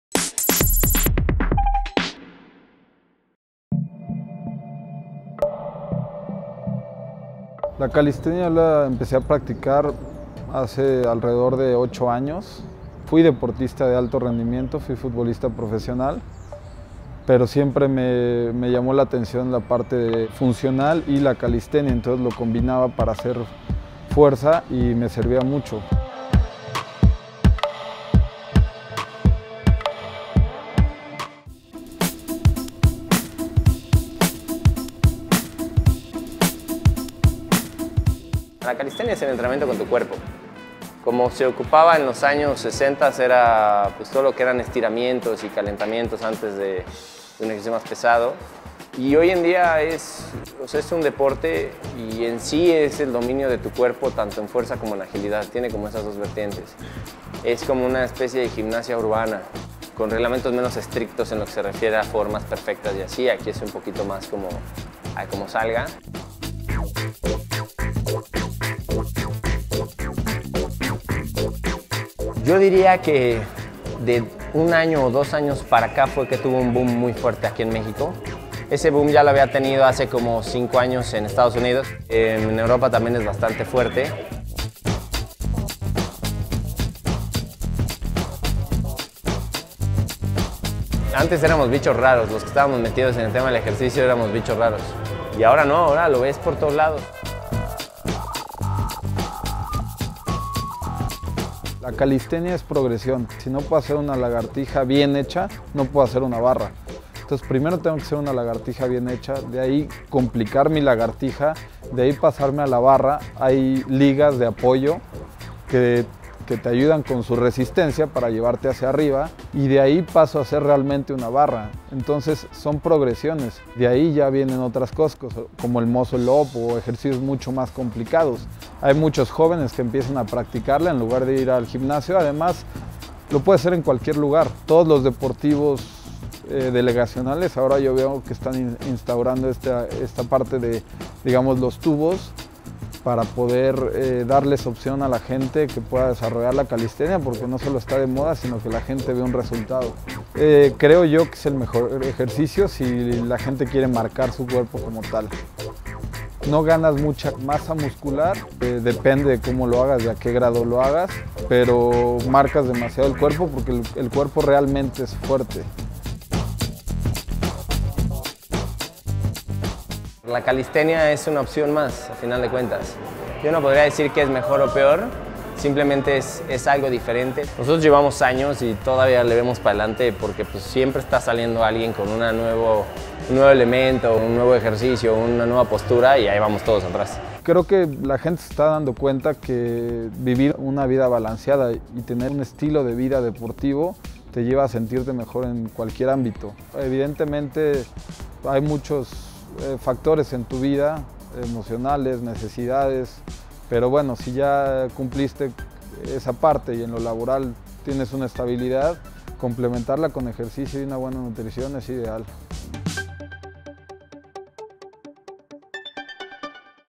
Calistenia: una alternativa para cuidar y fortalecer el cuerpo
La calistenia es el entrenamiento con tu cuerpo, el cual ha adquirido popularidad en los últimos años entre jóvenes tanto de México como a nivel mundial.